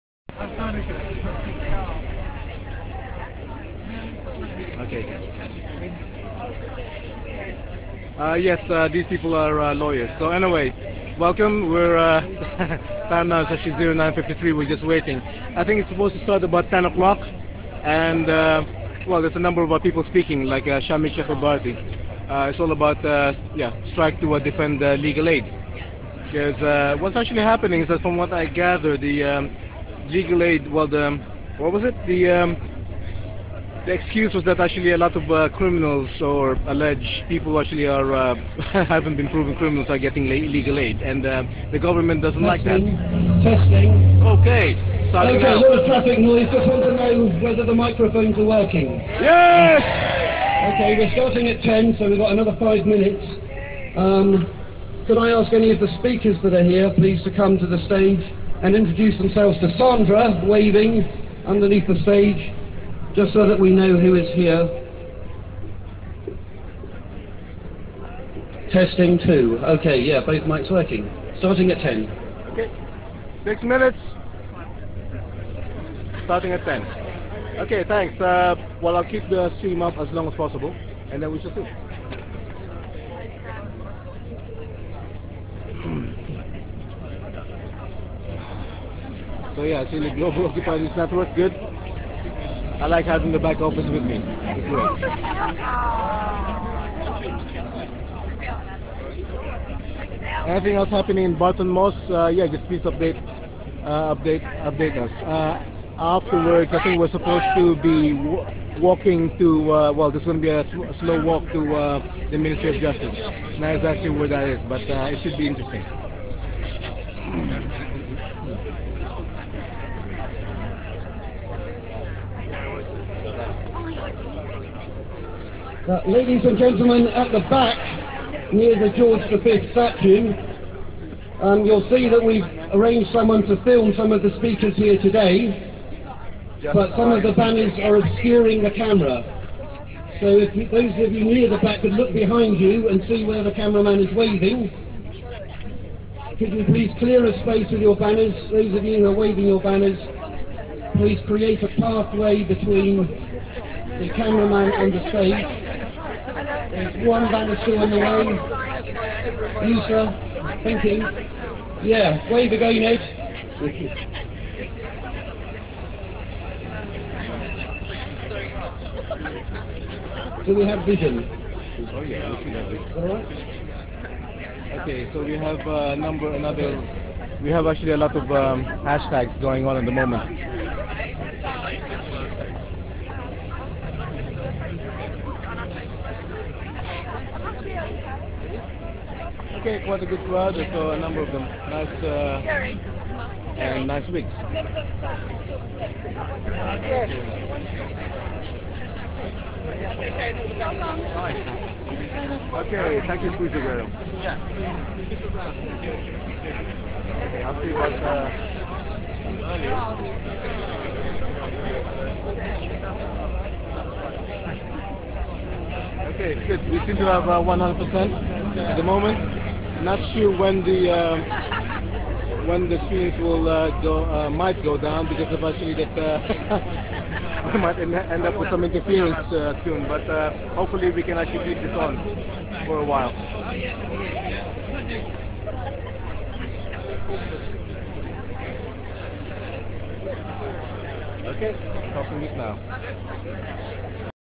LegalAid Demo Fight4LegalAid. Just4justice Part1 DTG070314
LegalAid Demo Fight4LegalAid. Just4justice olsx DTG070314.
Hundreds of Solicitors, Barristers have a strike due to the Grayling cuts to Legal Aid. Chris Grayling, First ever non-lawyer Lord Chancellor in 400 years.What is the price of Justice?